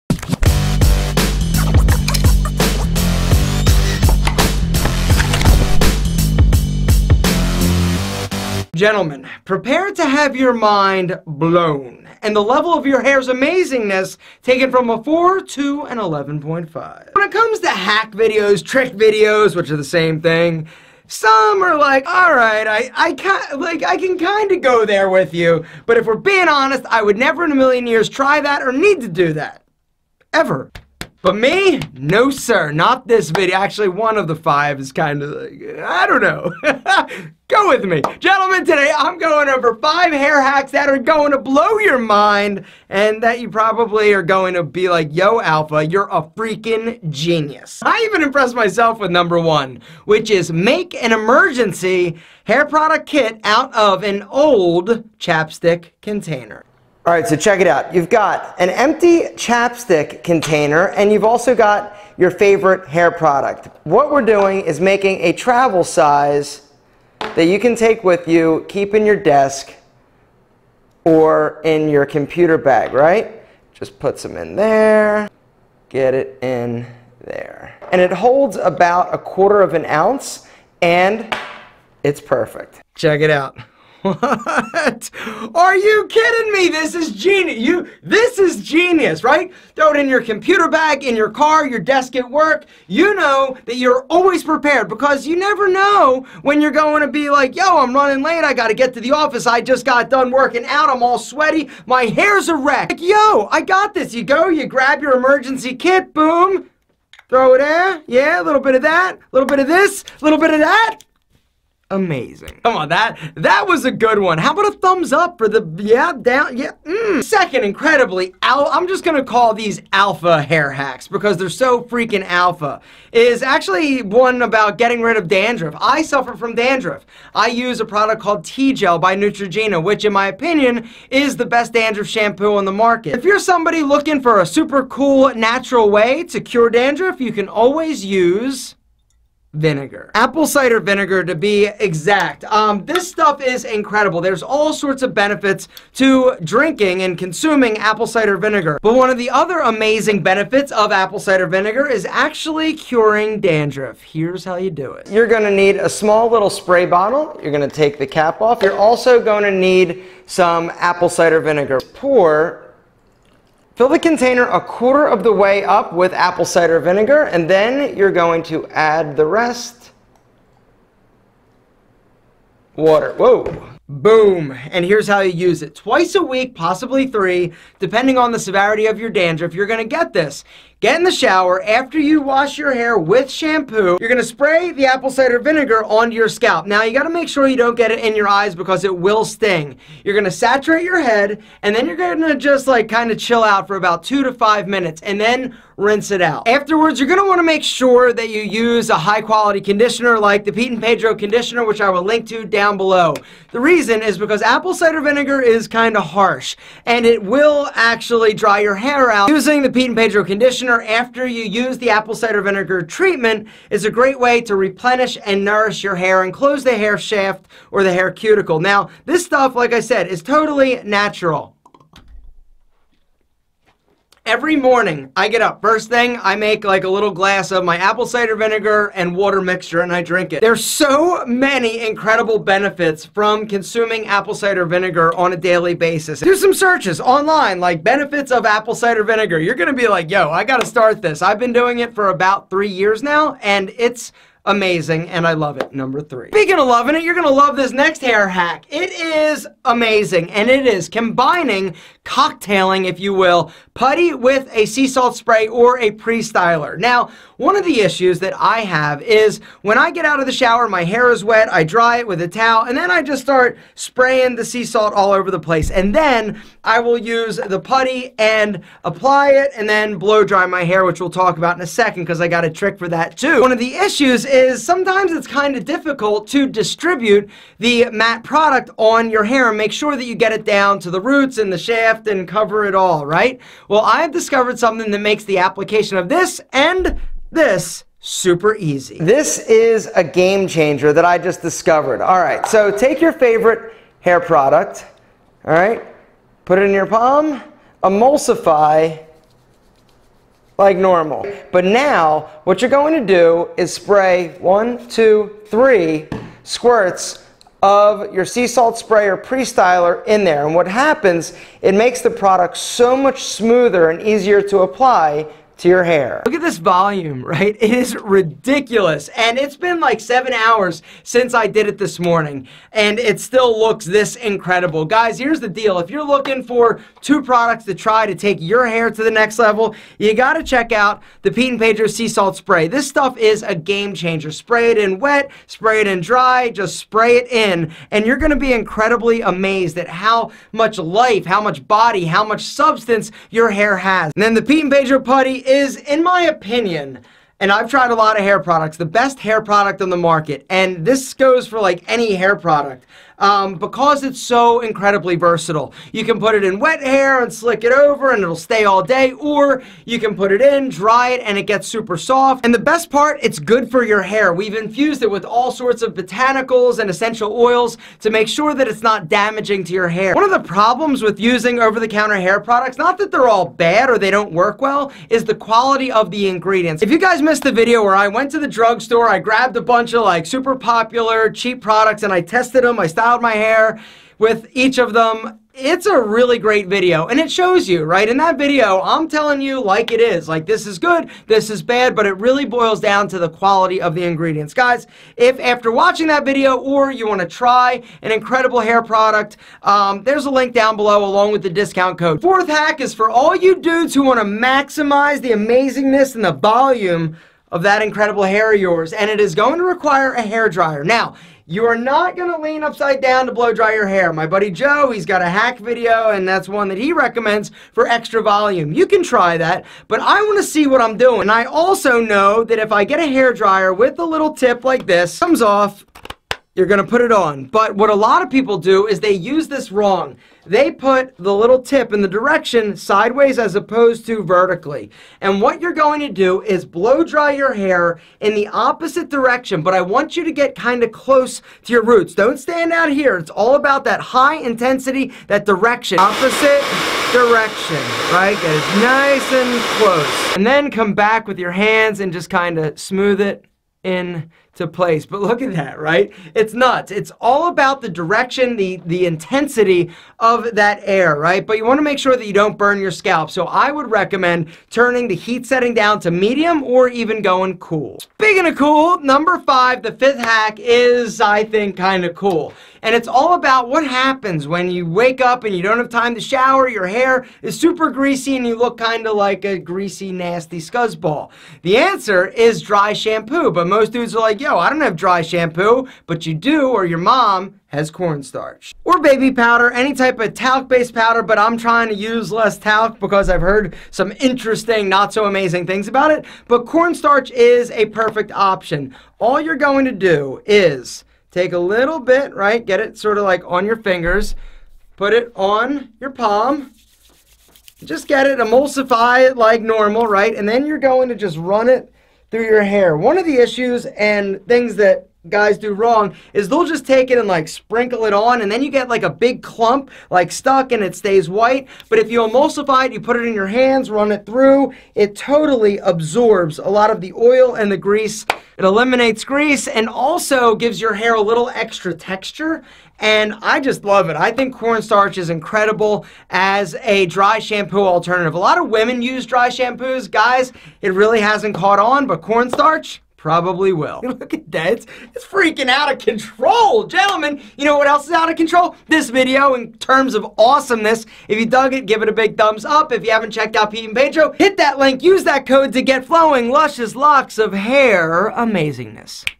TOP 5 Hair Hacks EVERY GUY SHOULD TRY!
Prepare to have your mind blown - your hair will be taken from 4 to an 11.5 in amazingness. In this video men's style, grooming, fitness and lifestyle expert, Aaron Marino of IAmAlphaM, AaronMarino, and Pete & Pedro  says some hack videos are not worth trying. But the hair hacks will make you think Alpha's a hair genius.
Must Do ALPHA Hair Tricks!
1. Make an emergency {travel size} hair product kit out of an old lip balm container

2. Use apple cider vinegar to cure dandruff - spray bottle + apple cider vinegar + water, use 2 - 3 times a week after shampooing, then condition

3. Putty + prestyler cocktail - emulsify Putty + 3 squirts of Salt

4. Maximize volume - blow opposite direction with medium heat

5.Greasy hair? Use corn starch or any talc powder